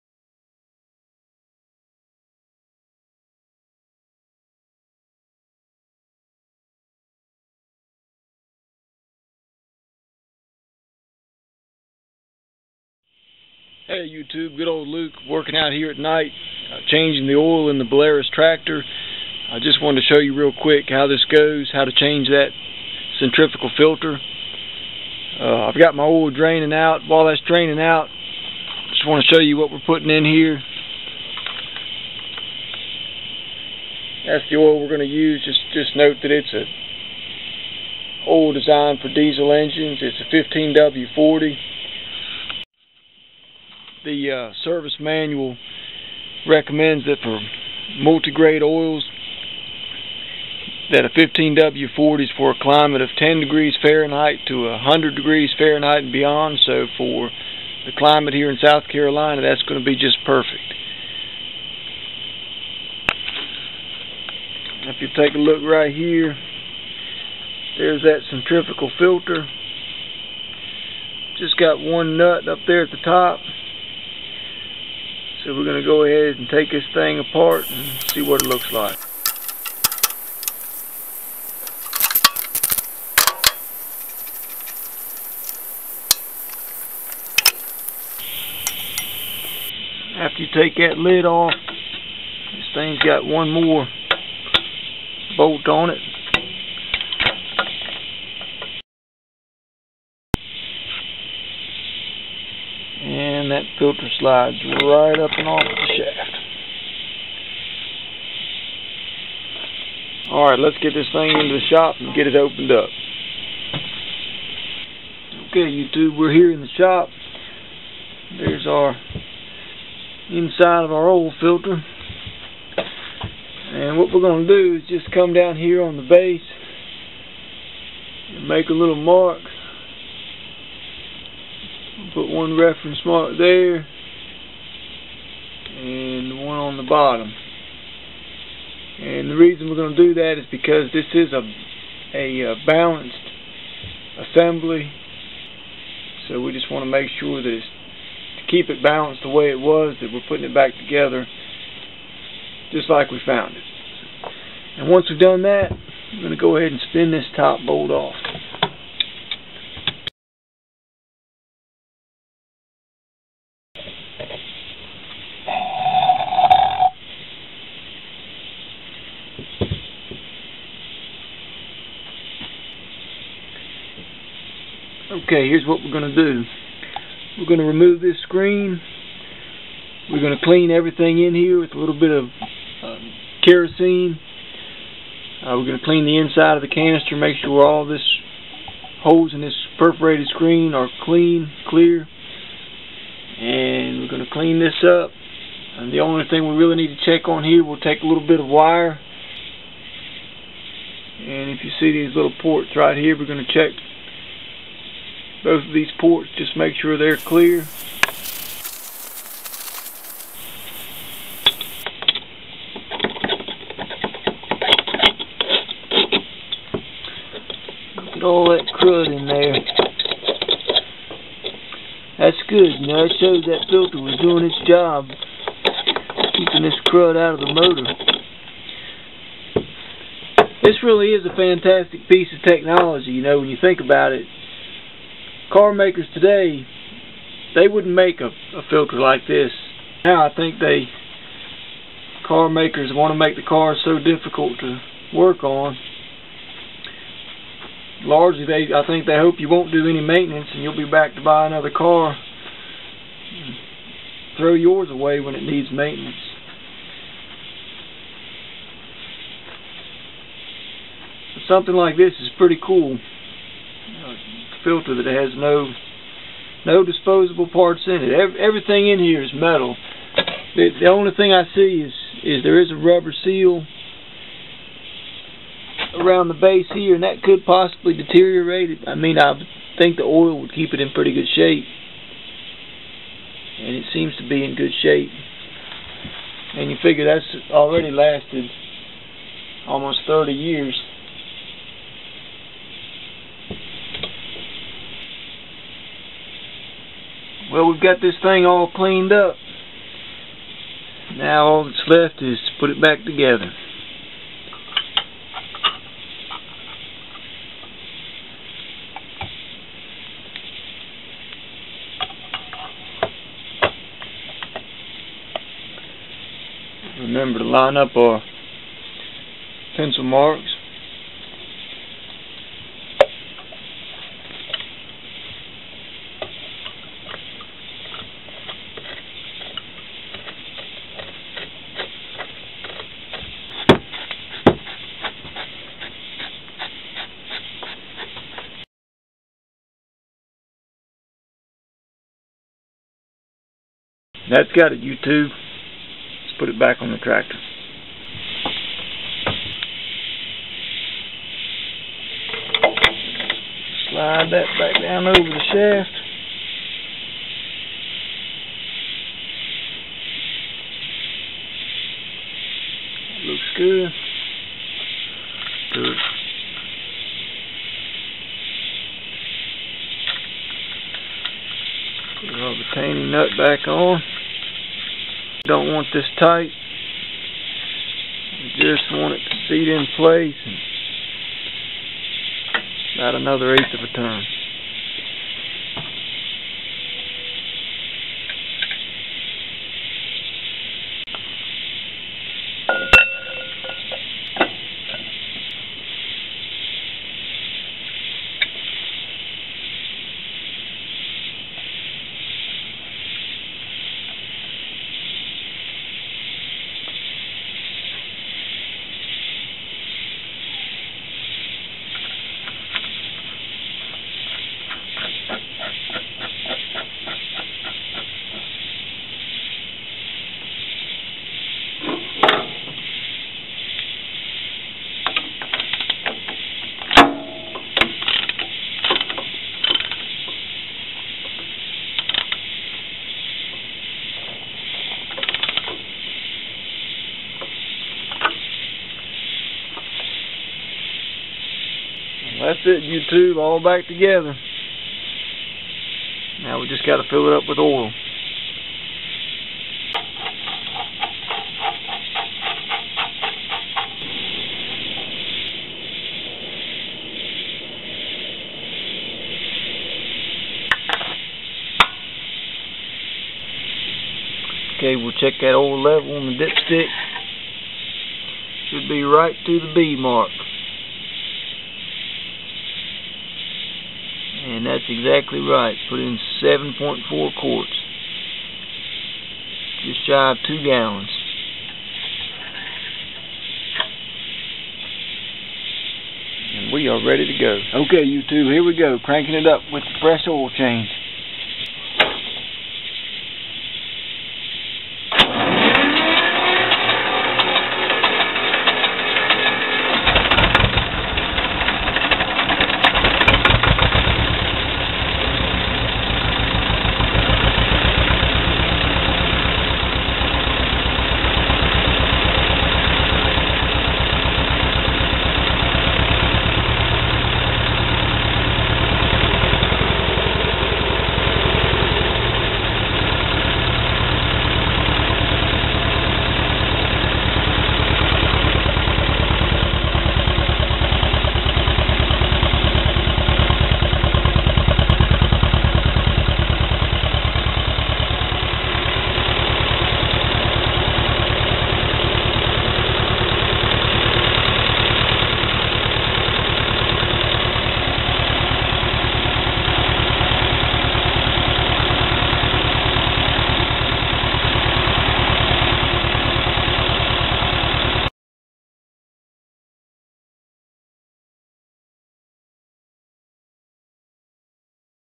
Oil Filter Tech on the Belarus 250AS Tractor
How to remove, service and replace the oil filter on the Belarus 250 AS.  This filter has no disposable parts and was designed for rural owners who could not run to the dealer every time their tractor needed service.